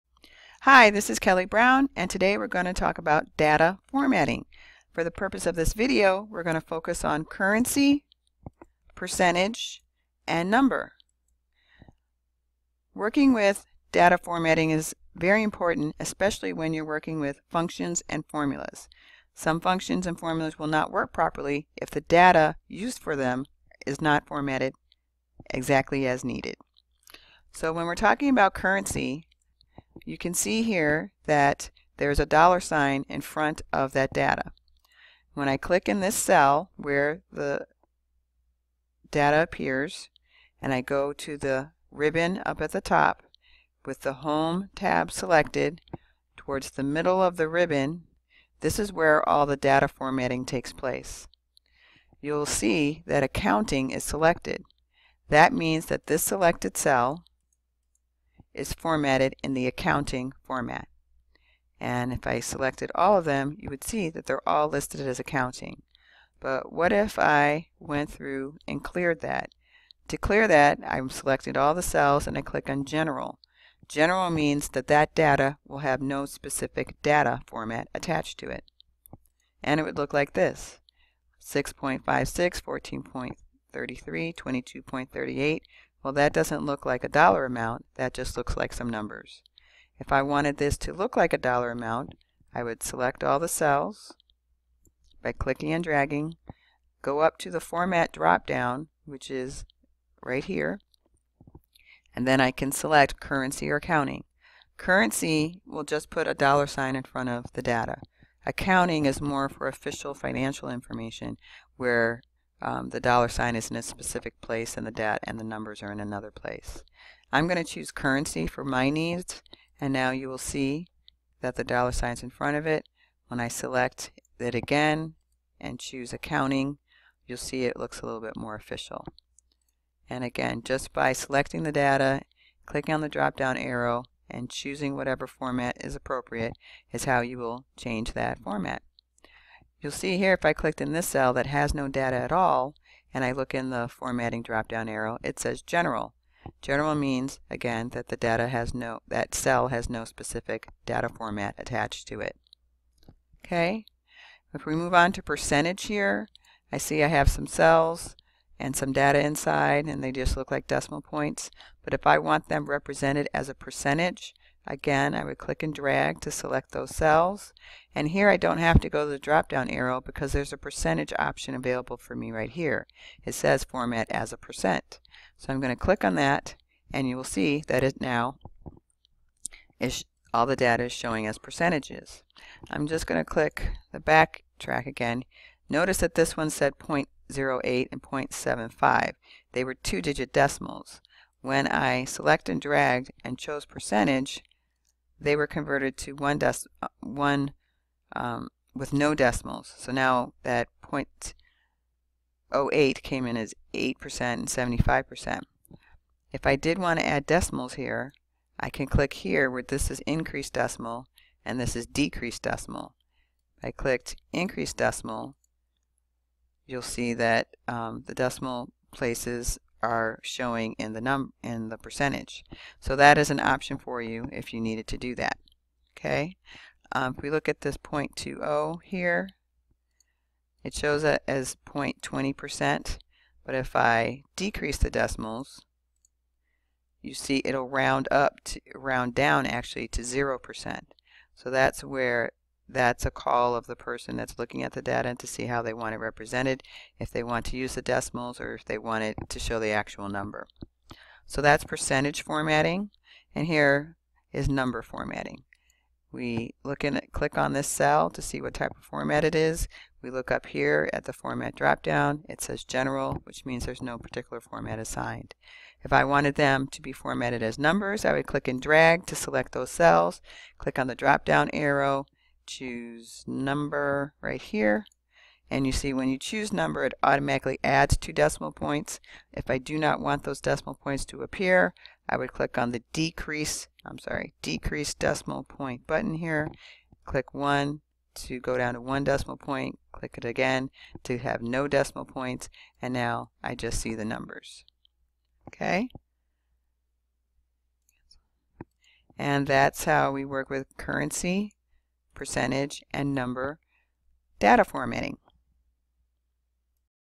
Currency, Percentage, and Number Data Formatting
This video demonstrates how to set data formatting for numbers, currency, and percentages in Excel.